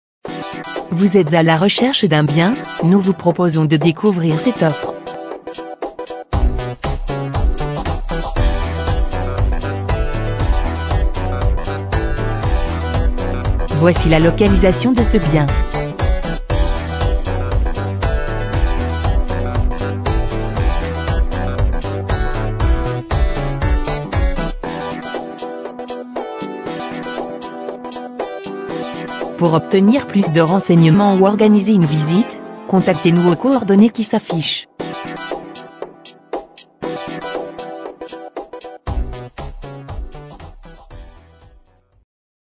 A louer - Appartement - Marseille 13eme (13013) - 4 pièces - 68m²
A découvrir ce bel appartement de type 4 lumineux refait à neuf composé d'un salon séjour donnant sur beau balcon avec vue dégagée orientation ouest , d'une cuisine aménagée indépendante,de 3 belles chambres dont 1 dressing,d'une salle d'eau et wc indépendant.Double vitrage et climatisation réversible dans tout l'appartement.Résidence sécurisée et entretenue,recherchée pour son coté familial,calme et proche de toutes commodités.Idéal famille! (MM) Agent commercial indépendant du réseau national clairimmo Maxihome N° RSAC : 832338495